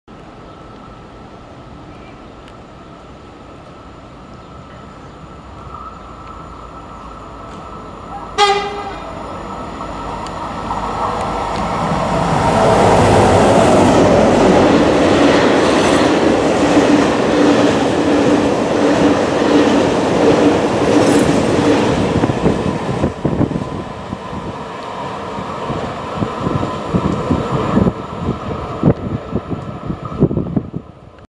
Takargo Rail 6000 loco 6003 + Timber Wagons passes Curia Station
Date: 9 June 2016
Time: 16.42
Line: Linha do Norte